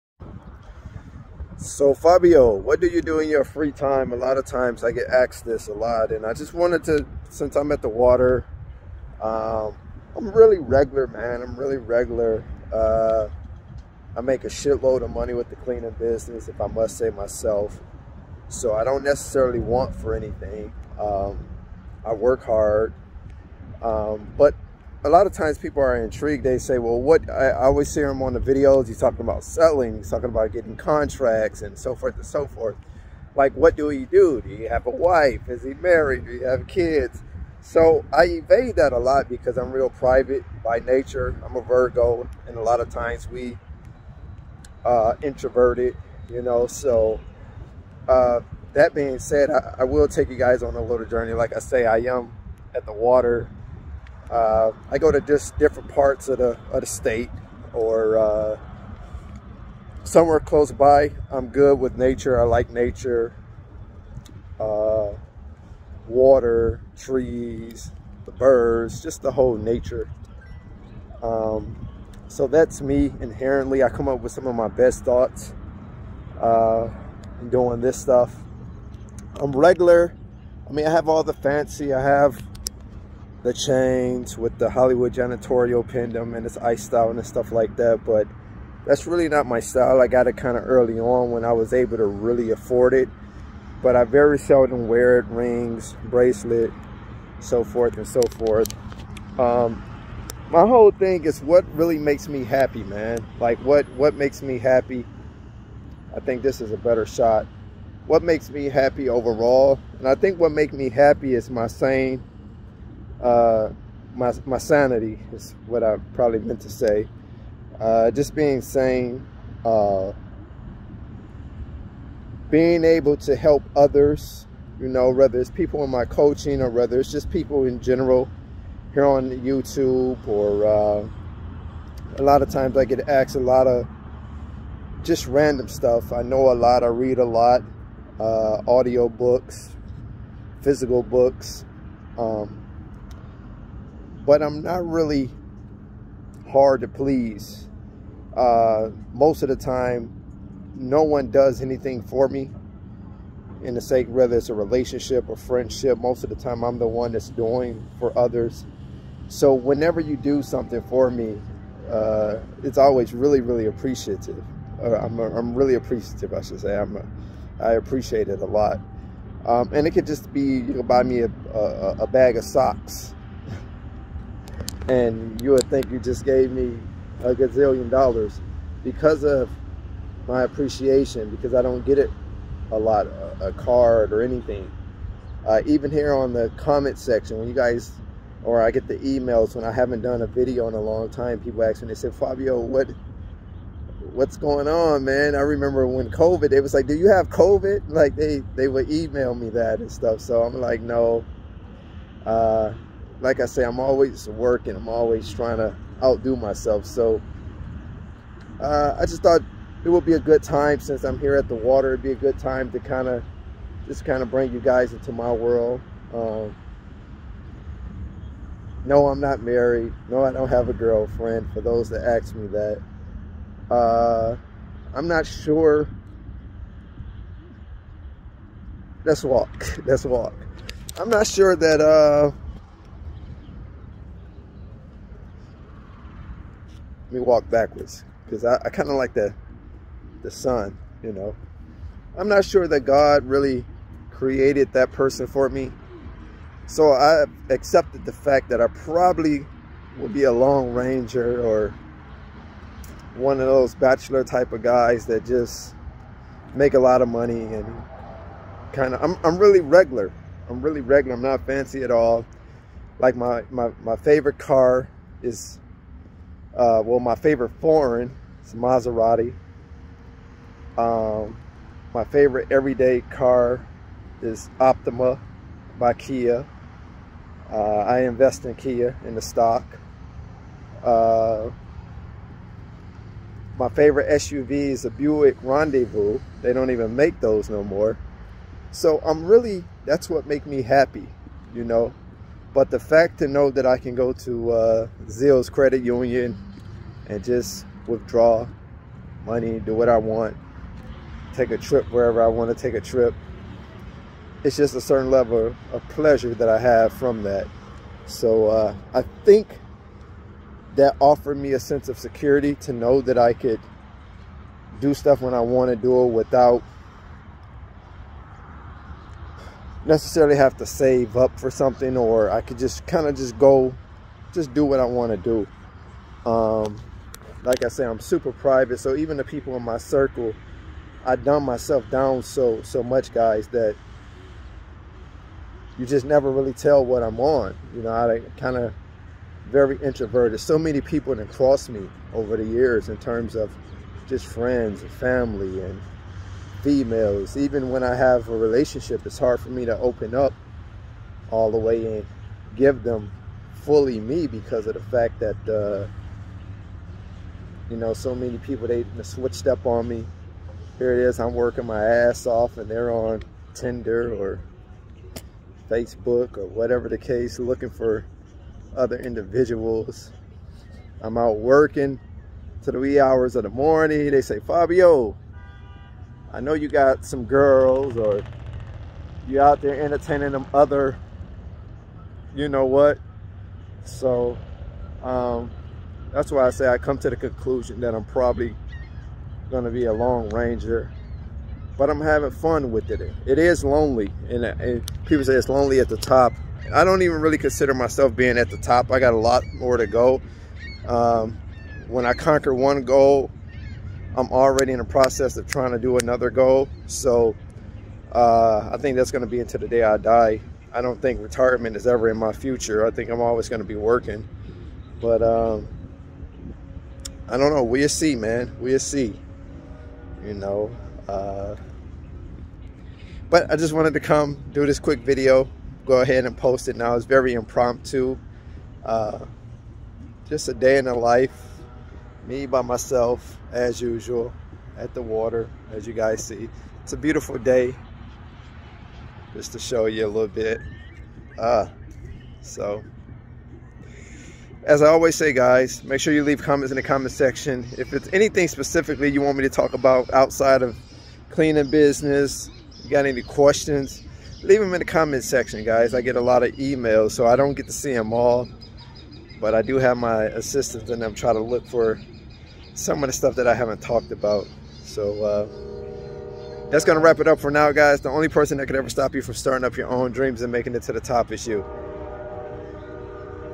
DAY IN THE LIFE WITH FABIO PART 1 😊🔒👍👏👌💪
ALOT OF TIMES IM ASKED ABOUT MY PERSONAL LIFE. SO I DECIDED TO SPEAK ON IT BRIEFLY